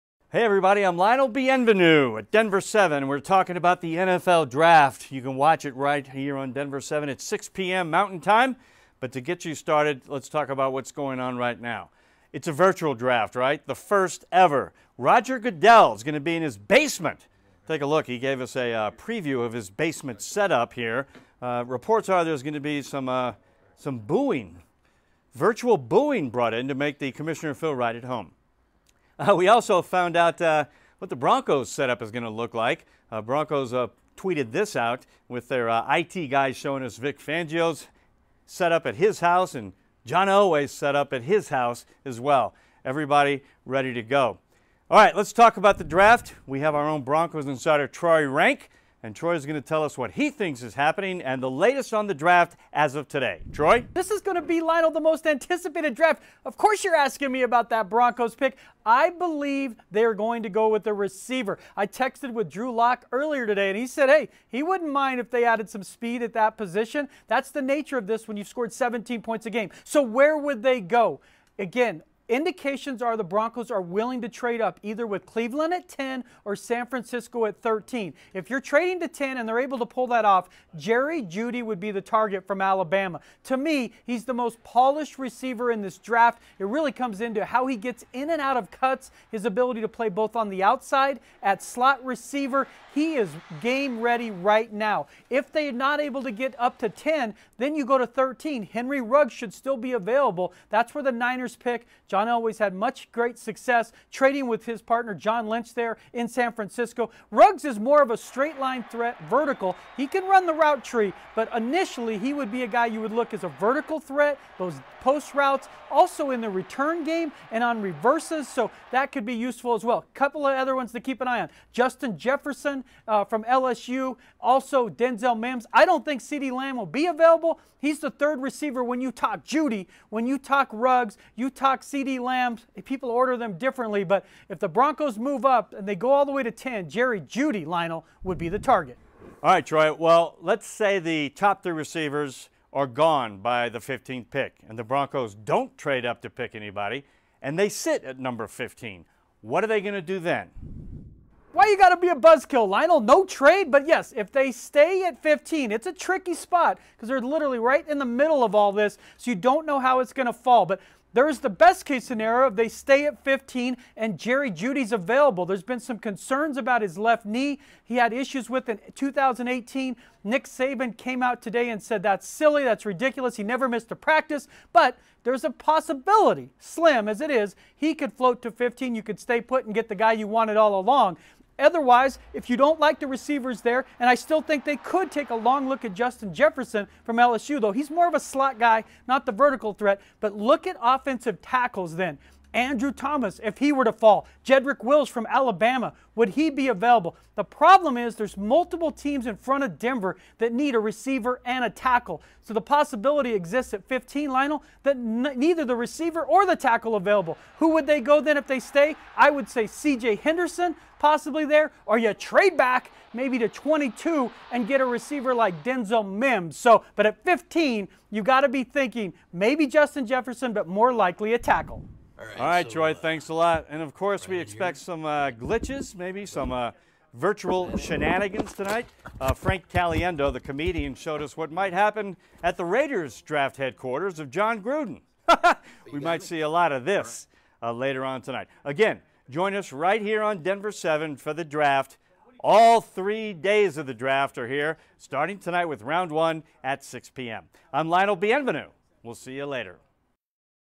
Trade up, or stay at No. 15? Who the Broncos should take in both scenarios
Lionel Bienvenu and Troy Renck break down the best-case scenario for the Broncos in Thursday's first round of the NFL Draft.